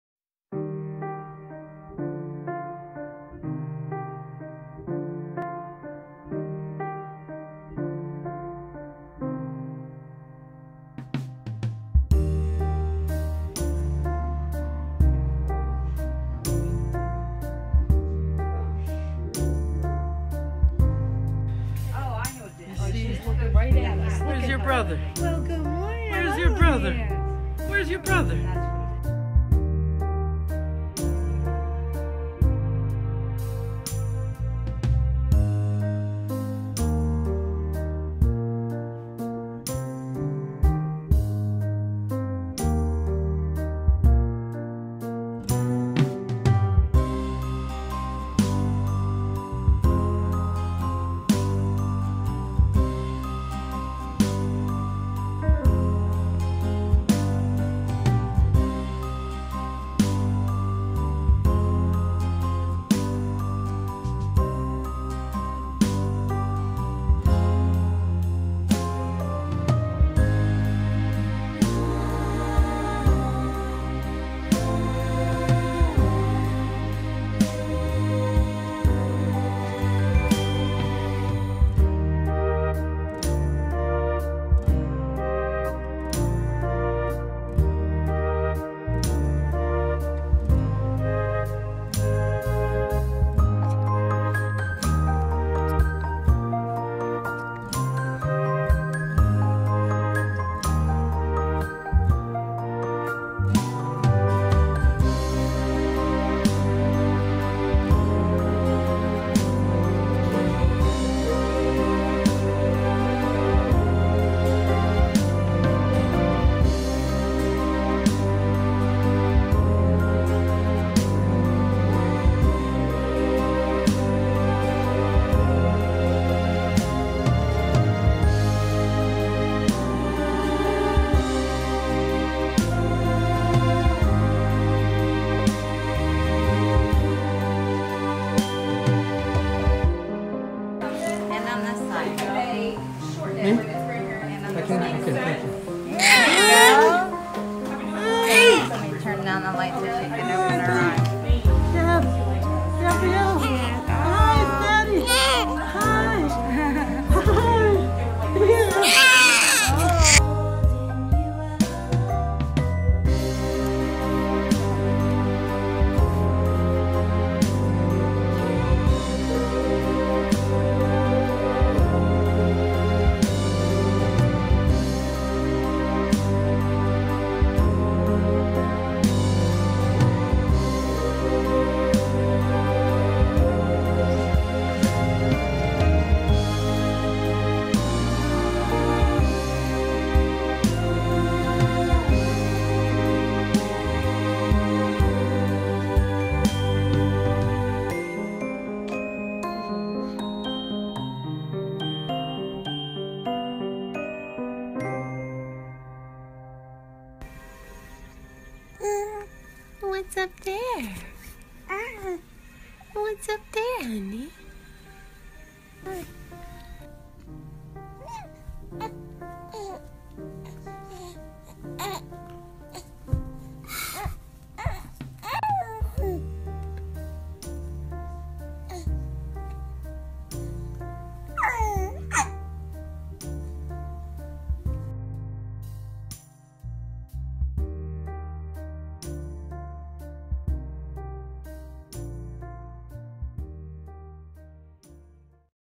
Dear Gabrielle T
Now three months old, I dedicate this video to the precious girl who changed my world. I thank God for filling a piece of my heart that I never knew was missing.

This is my song to you, sweetheart.
We all love you so much.